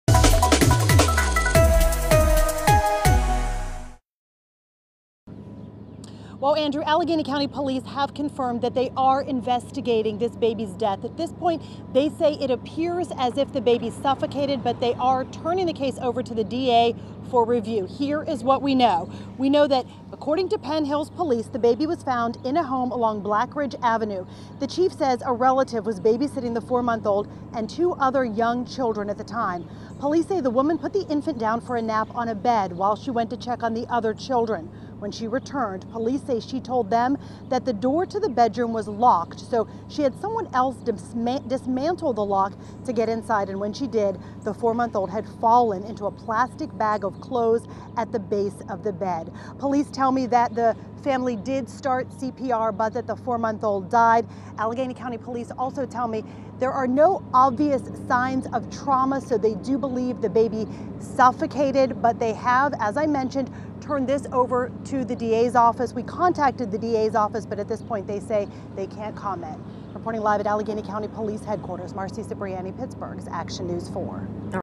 Infant dies after falling in to plastic bag
marcie 5 dead baby